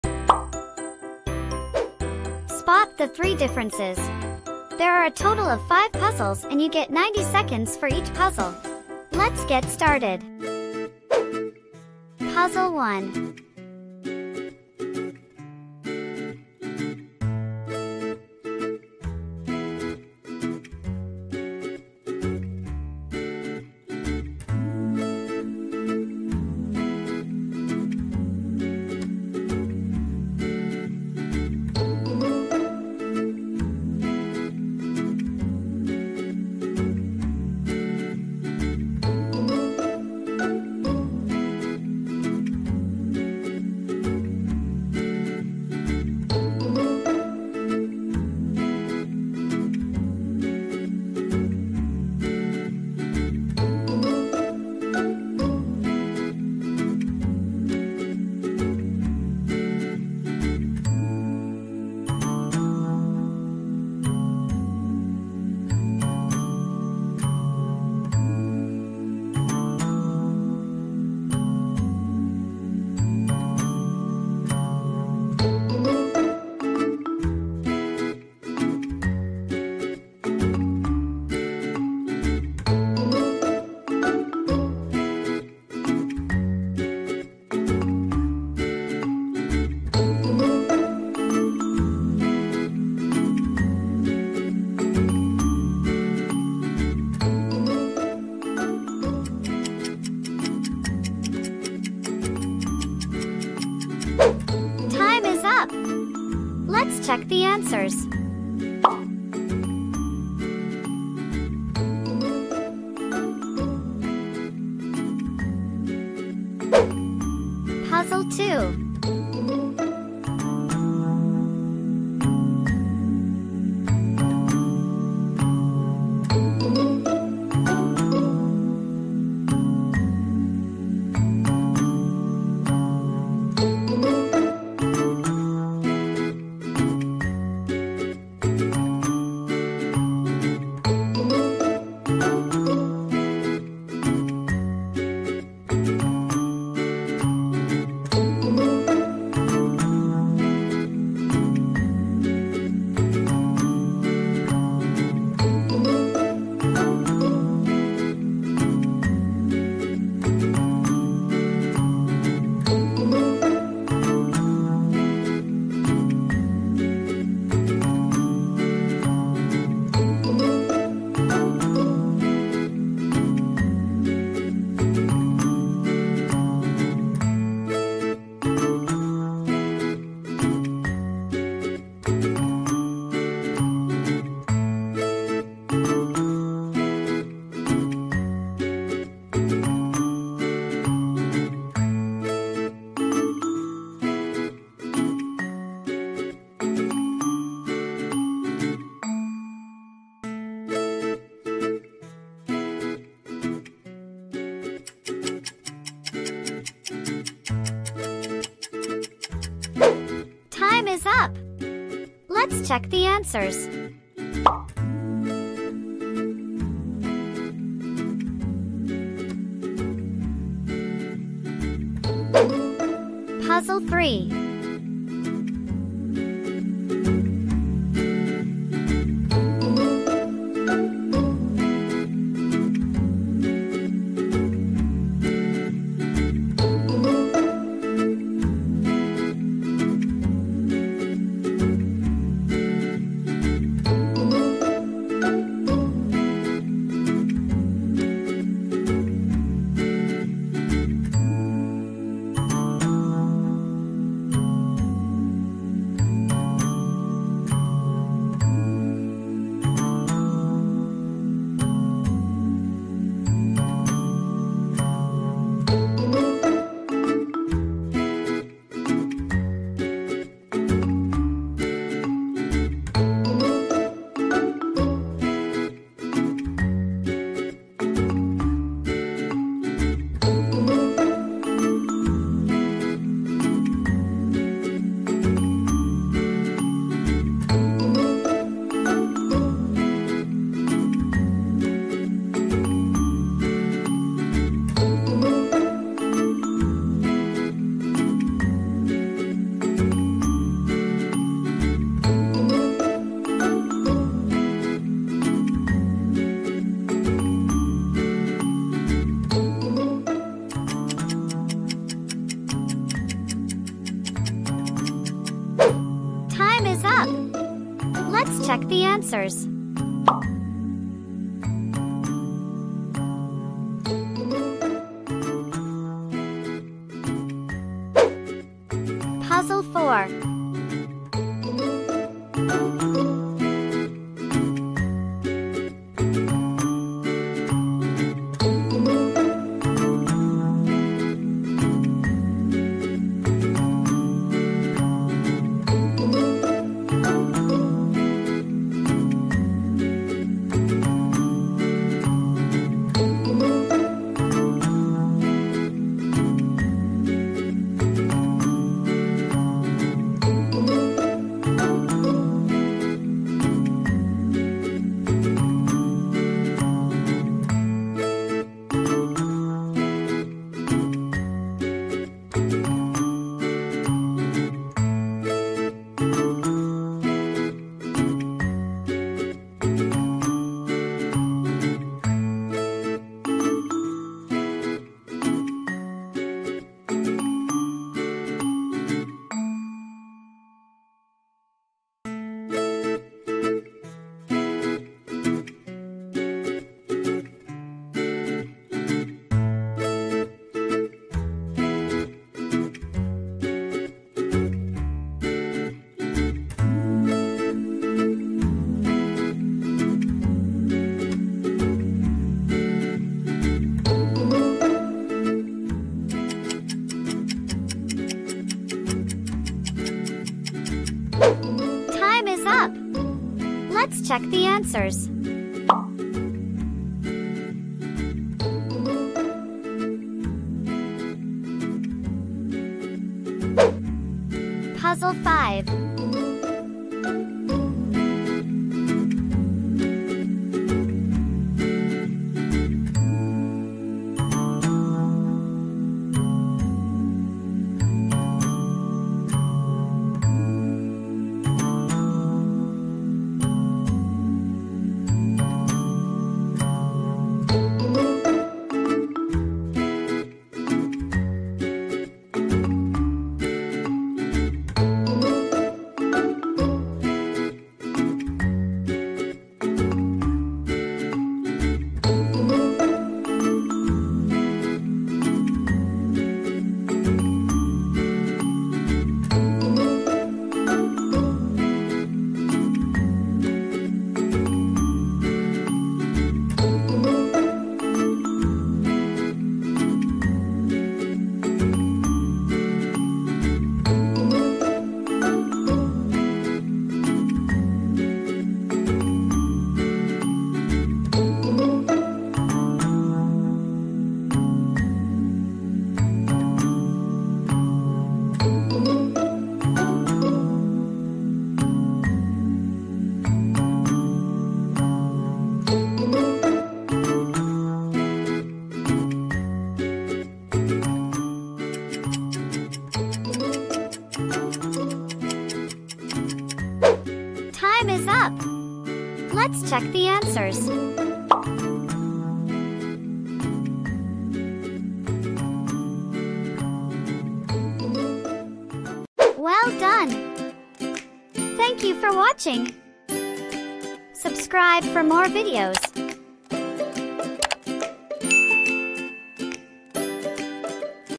Spot The Difference : Only Genius can Find It? [ Find The Difference #6 ]
Welcome, dear friends, to a game that hones your detective skills – Spot the Difference.

This intriguing activity isn't just about identifying distinctions; it's a mental exercise that refines your concentration, cognition, memory, and observation.
Moreover, engaging in this game offers more than just fun; it's an ally in the battle against cognitive decline, potentially preventing dementia by sparking cognitive activity.

Within the next 90 seconds, your task is to locate three discrepancies between a pair of pictures.

Your support fuels our dedication to crafting engaging content. Share your progress in the comments section – how many differences did you manage to pinpoint?

We extend our gratitude to each of you who've joined us on this cognitive journey.

Timeline:
0:00 Introduction
0:10 Puzzle 1
1:56 Puzzle 2
3:42 Puzzle 3
5:28 Puzzle 4
7:13 Puzzle 5